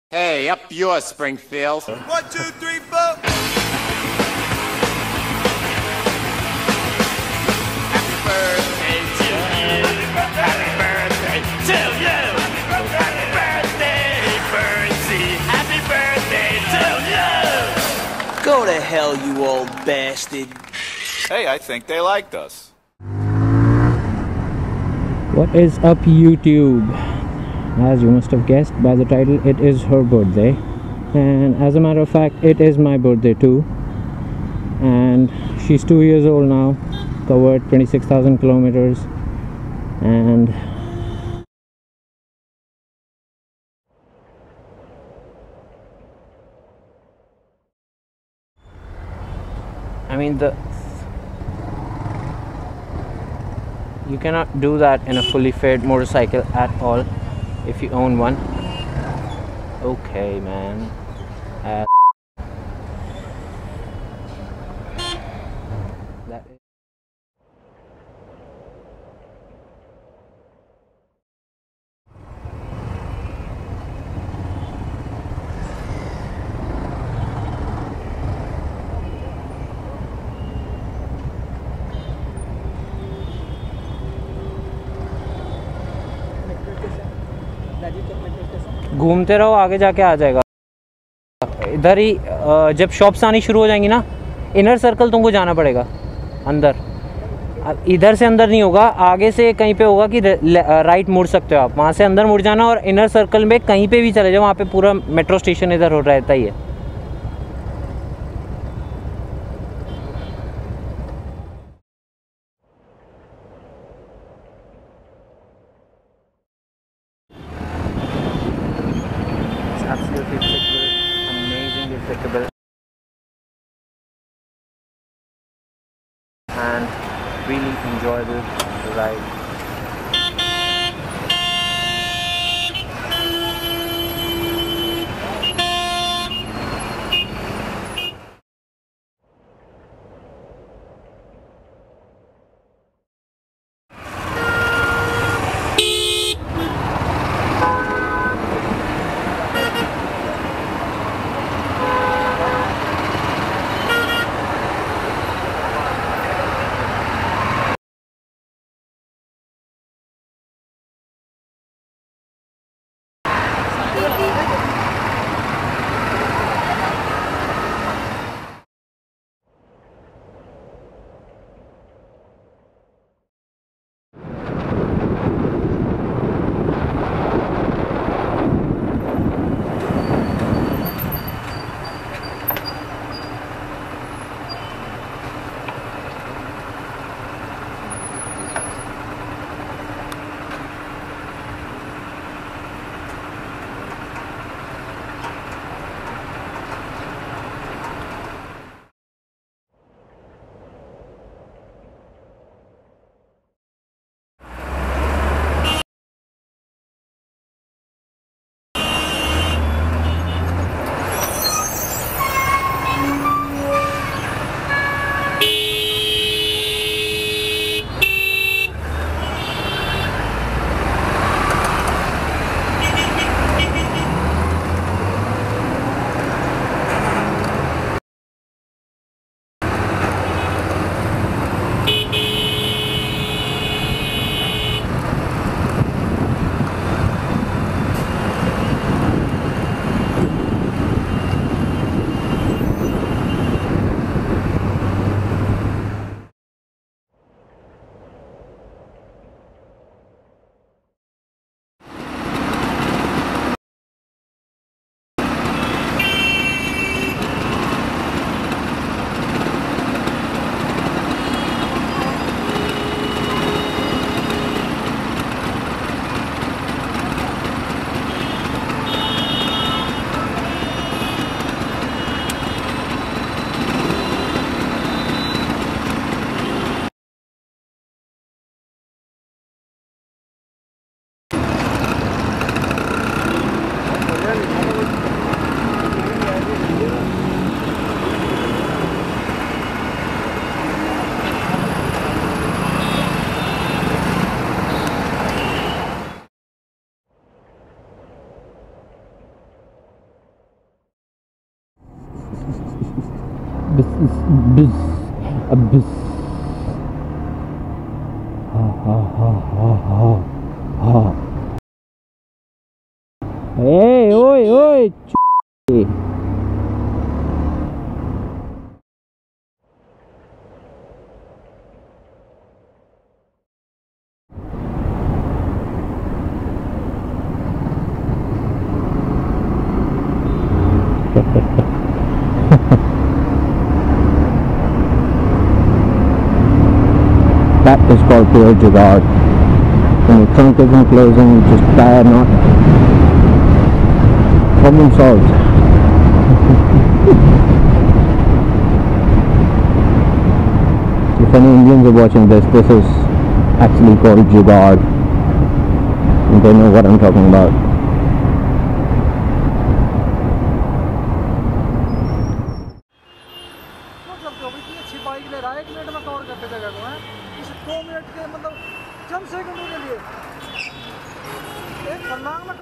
Yamaha YZF R15 v2.0 - Episode 15 : ★★Birthday Special★★
A special video as she turns two on 24th July. Spent whole day with her, brought her a cake, gave a nice wash + wax and etc etc.
She was so happy she made me race with a cbr and won. Too bad I can't post it on youtube coz of legal reasons. But hey, enjoy the rest of the observations :)